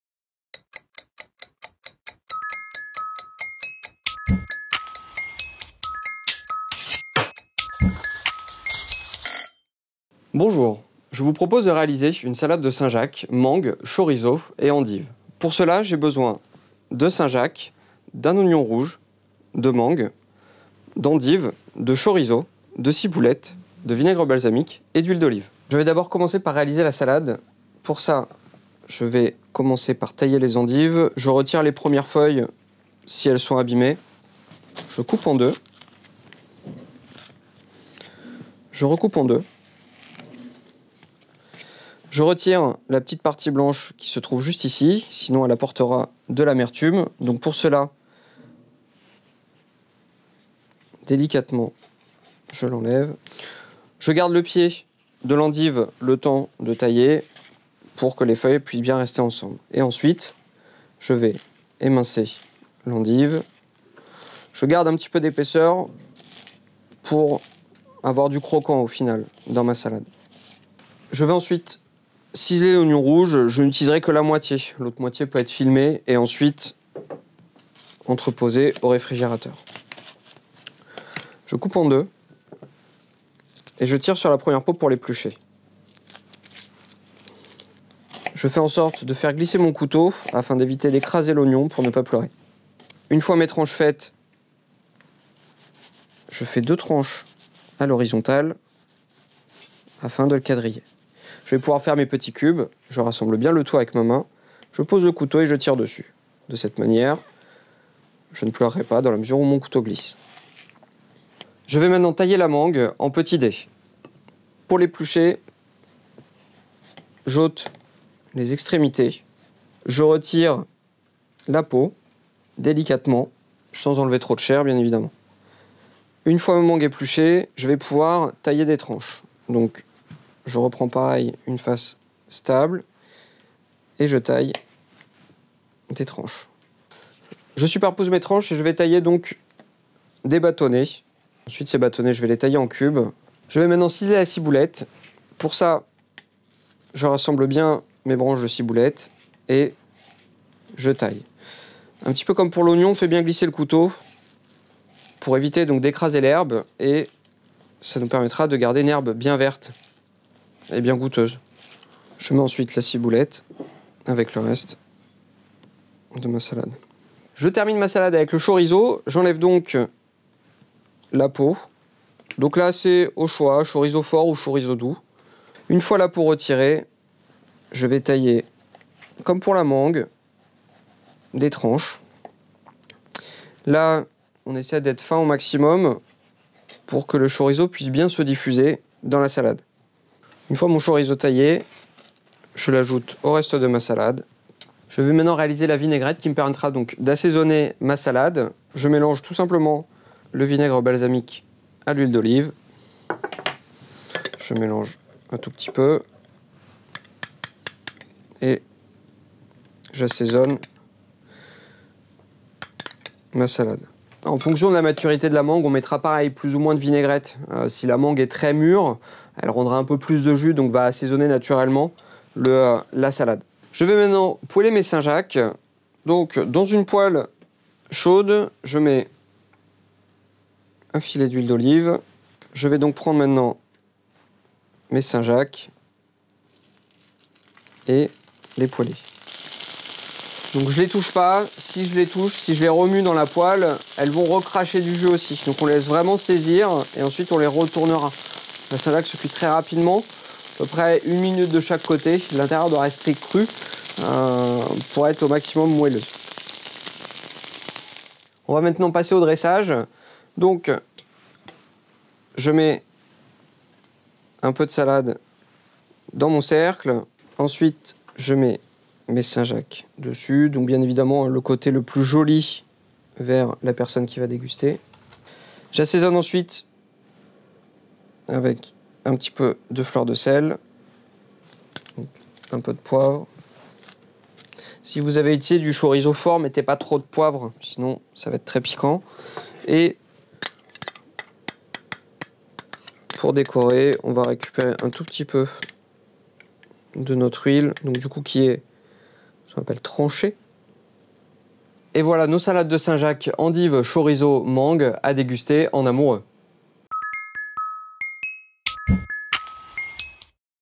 Recette Saint Valentin de Salade de noix de Saint-Jacques, endives, mangue et chorizo
Recette en vidéo de L'atelier des Chefs

Une salade d'endives légèrement piquante et sucrée, agrémentée de noix de Saint-Jacques poêlées.

    Noix de Saint Jacques : 18 pc(s)
    Endive(s) : 4 pc(s)
    Chorizo : 100 g
    Mangue(s) : 200 g
    Oignon(s) rouge(s) : 1 pc(s)
    Ciboulette : 0.5 botte(s)

    Vinaigre balsamique : 5 cl
    Huile d'olive : 8 cl
    Sel fin : 6 Pincée(s)
    Moulin à poivre : 6 Tour(s)

Retrouvez cette recette en vidéo en cliquant ainsi que toutes les vidéos des Chefs de L'atelier des Chefs